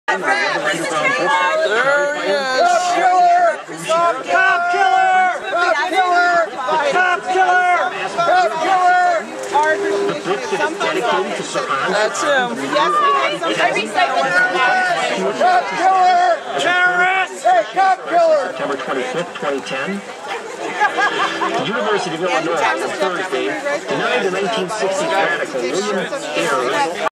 Bill Ayers
Unrepentant terrorist / cop killer, William Ayers greets liberal school reform activists in Milwaukee, at Stonefly Brewery on  Oct. 15, 2011.

We gathered to let this group know that we still remember the officers killed and bombs planted by Ayers and his group, The Weather Underground.